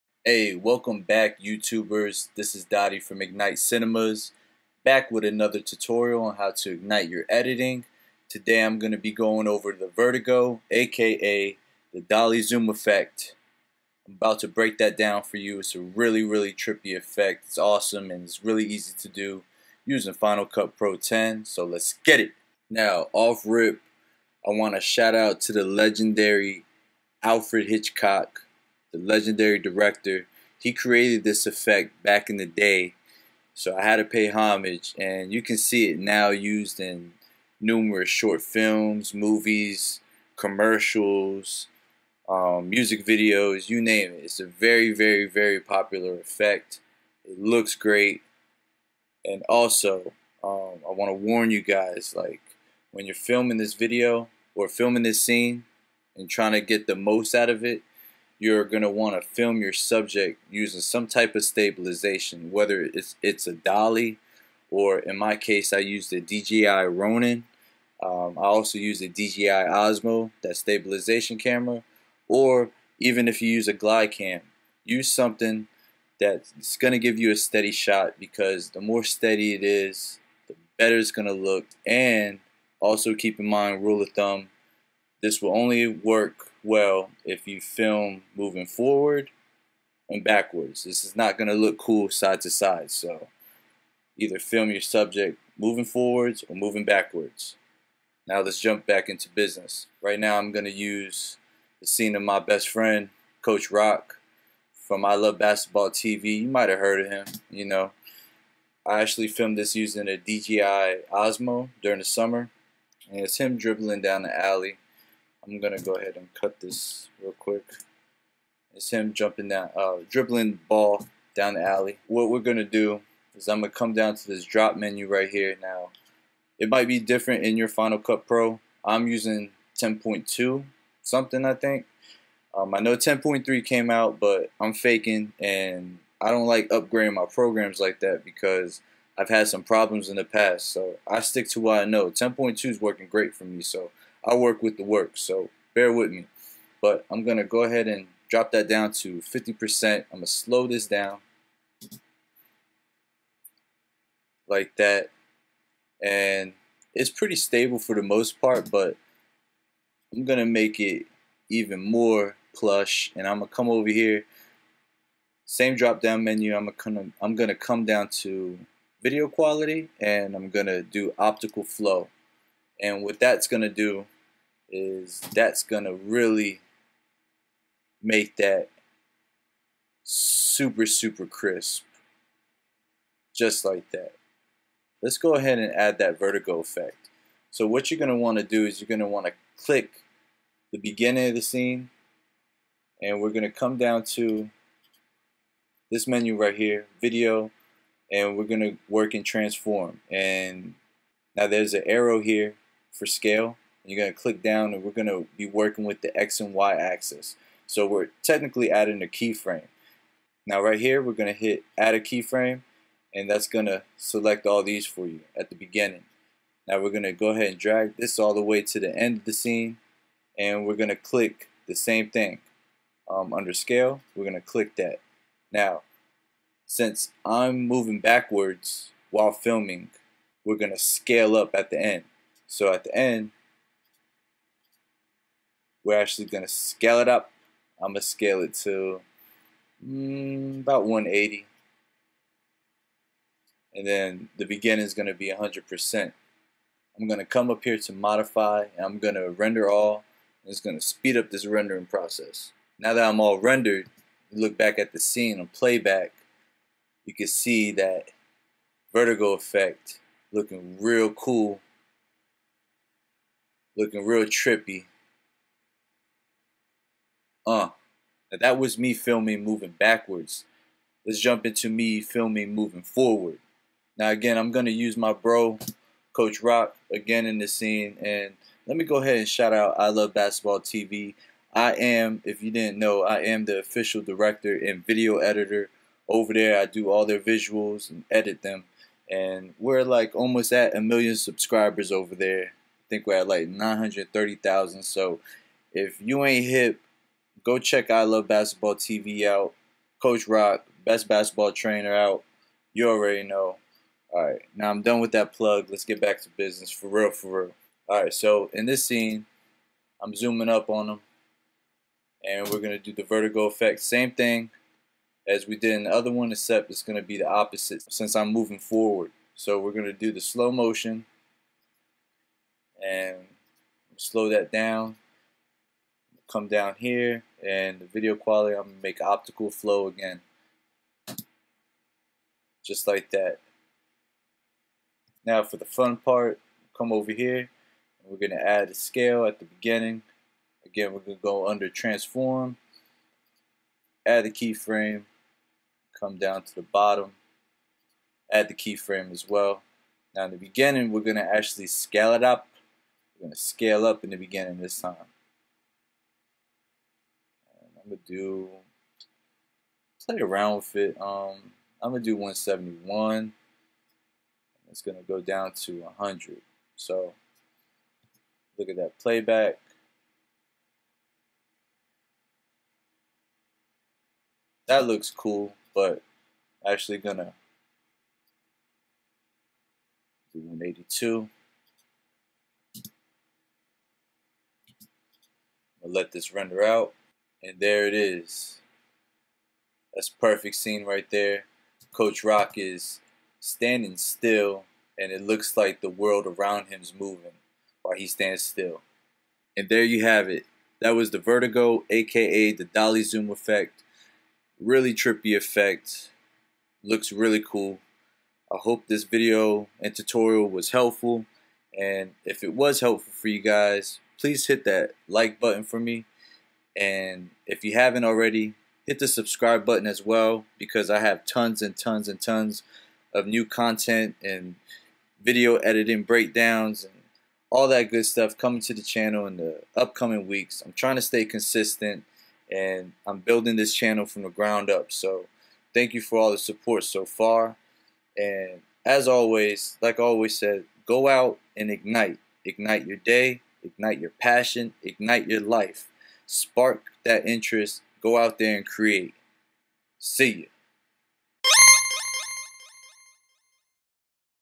Vertigo aka Dolly Zoom Effect Tutorial in Final Cut Pro X
Vertigo (Dolly Zoom) Effect Tutorial in Final Cut Pro X
This week I show you step by step how to do this famous effect created by Alfred Hitchcock.
and don't forget to go out & IGNITE !